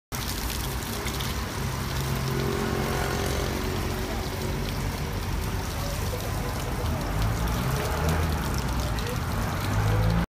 Secando o Carro com ÁGUA - Efeito hidrorrepelente Ceramic PRO 9H
O que é CERAMIC PRO? É um revestimento cerâmico protetor 9H, que proporciona resistência a riscos, muito mais brilho e proteção química avançada.

CWB Detail é aplicador oficial Cercamic PRO em Curitiba e região.

Trabalho realizado por CWB Detail

How to apply Ceramic PRO? What is Ceramic PRO?
This video depicts every step required to safely coat your car with CeramicPRO